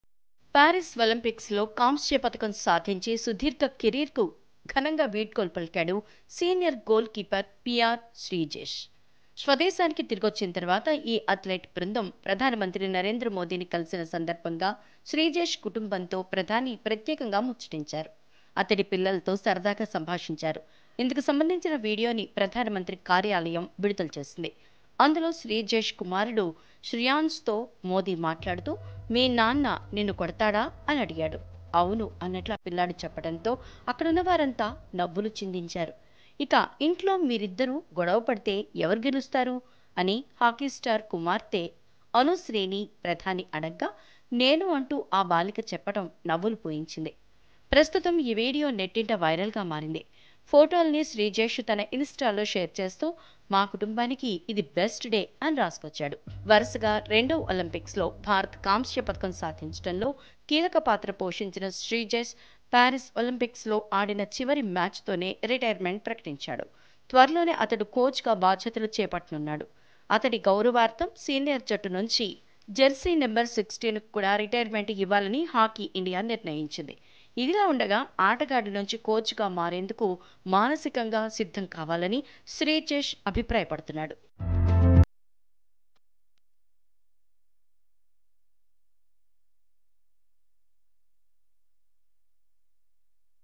శ్రీజేశ్ కుమారుడితో మోదీ ఫన్‌ చాట్‌..‘మీ నాన్న నిన్ను కొడతాడా?’ - TV9
Credits : sarada/ producer